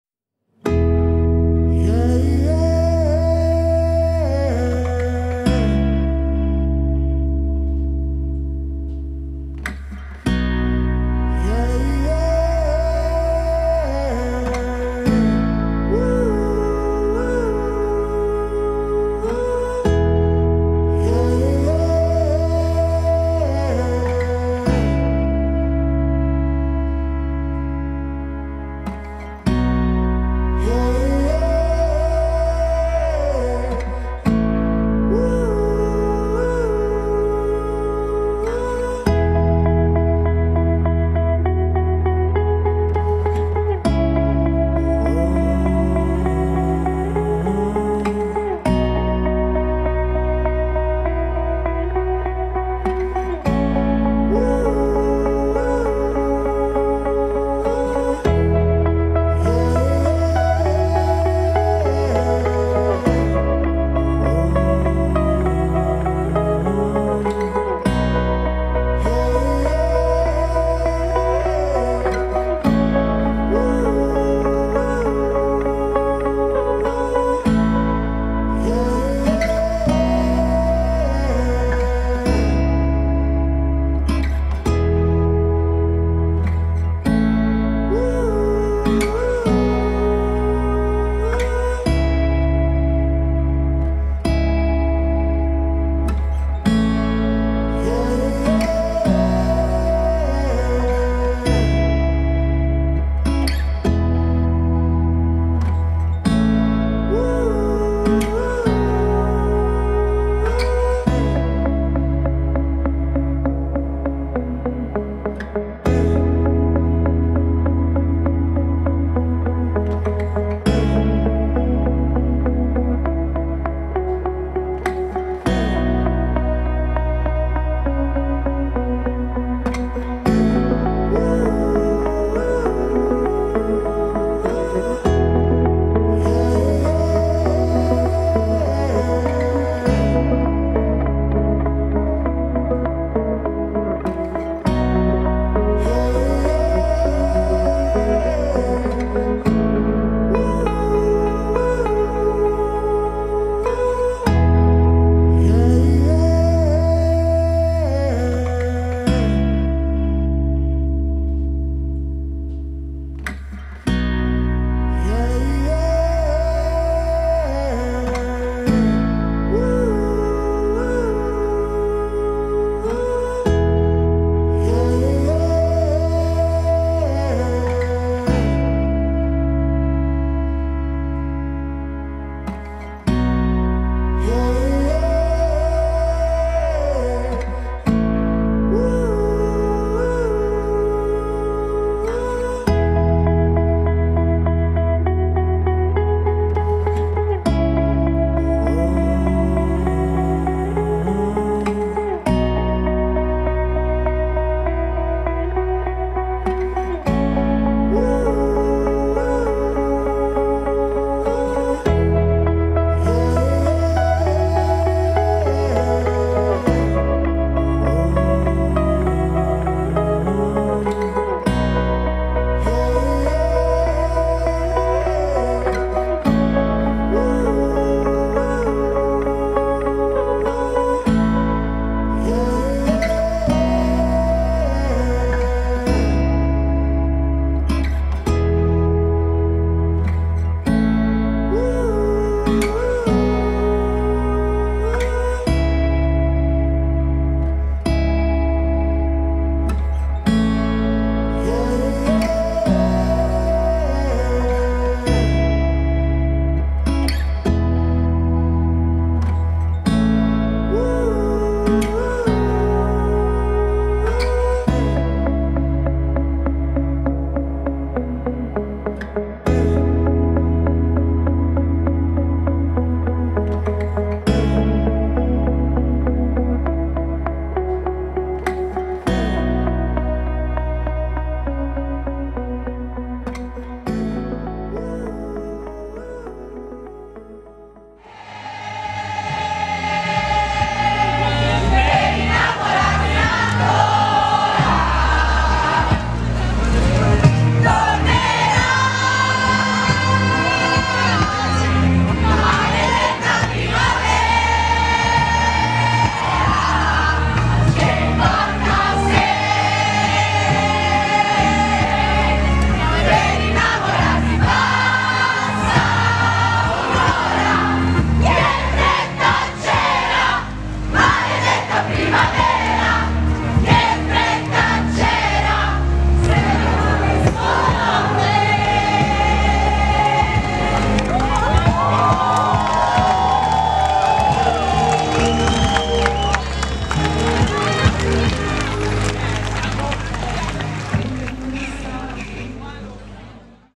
LODI PRIDE 2023
Sabato 1/07/2023 si è tenuto il primo Lodi Pride; la parata, partita in piazza Vittoria, si è conclusa in piazza Castello dove sono intervenuti vari attivisti, fondatori di associazioni e personaggi politici tra cui l'attuale sindaco di Lodi Andrea Furegato.